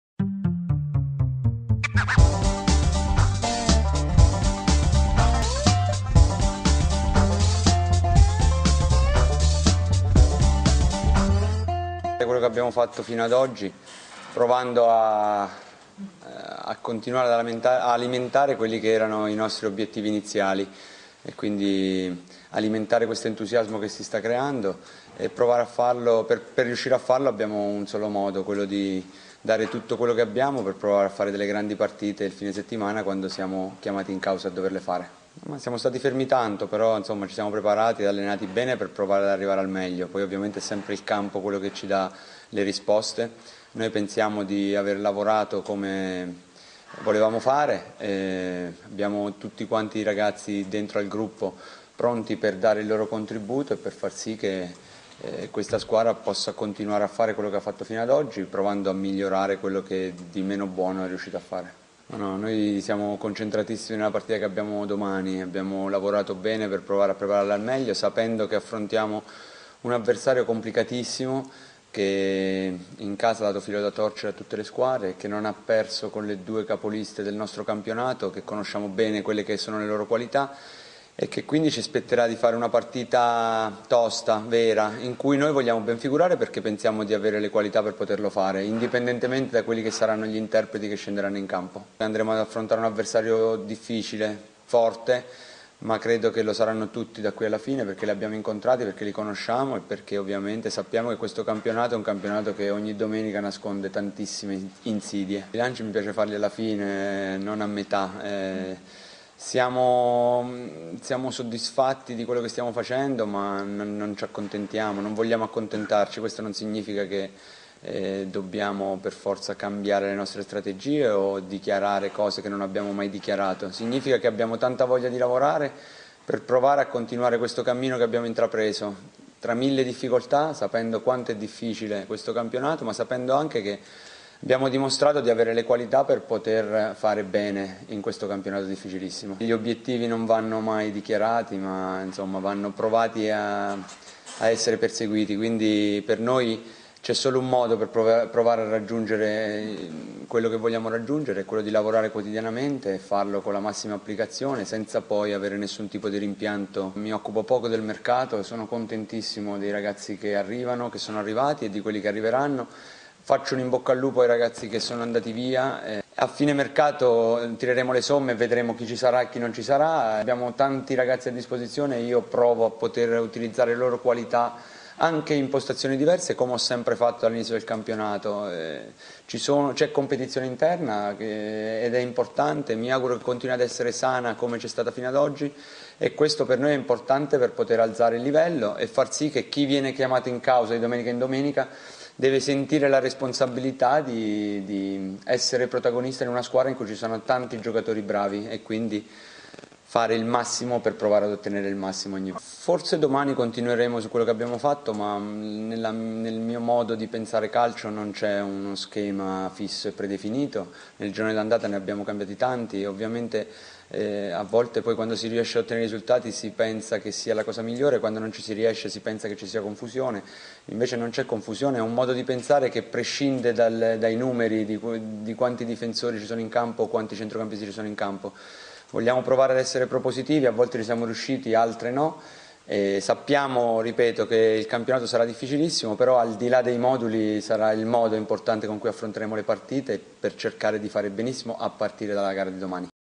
19/01/18 - Grosso: “Obiettivi stagionali e mercato, bilanci alla fine. A Cesena..."
Le parole del tecnico biancorosso in conferenza stampa ed alla vigilia della sfida del 'Manuzzi'